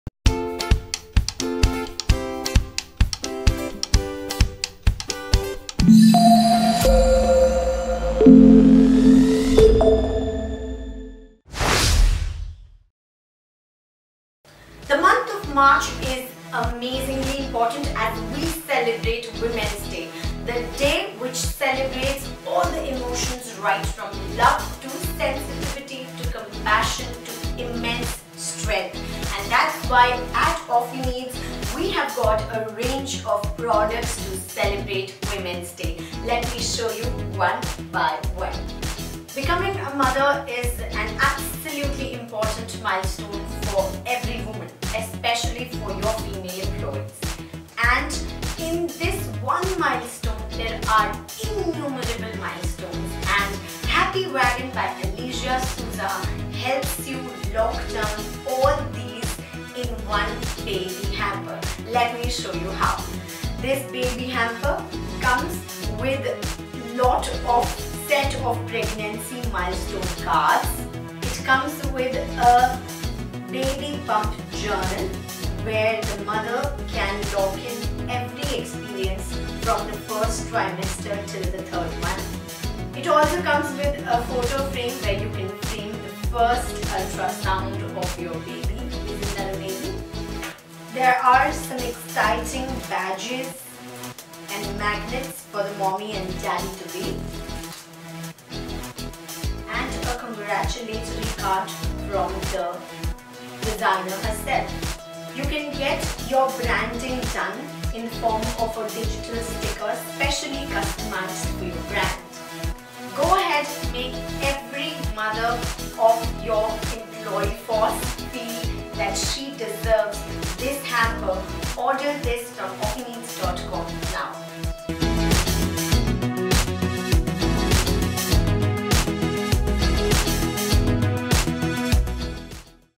Ep 750 Bump Box
Introducing the Bump Box - the perfect corporate gift for expecting mothers. This beautifully designed gift box is curated with essentials that expecting mothers need during their pregnancy journey. Crafted with utmost care and precision, the Bump Box features a collection of high-quality products that cater to the needs of pregnant women. It includes items such as stretch mark cream, a pregnancy journal, a body pillow, and other essentials that can make pregnancy more comfortable and enjoyable.